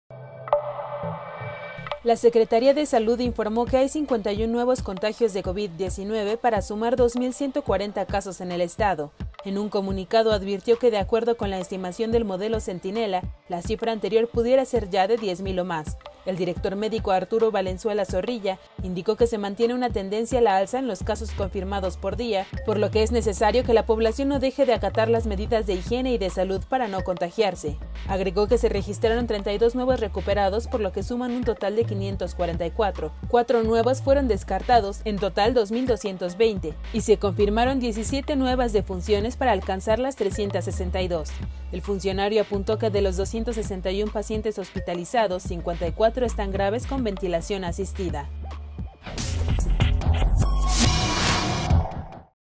Van 2 mil 140 casos de Coronavirus en Chihuahua
La Secretaría de Salud informó que hay 51 nuevos contagios de COVID-19 para sumar 2 mil 140 casos en el estado.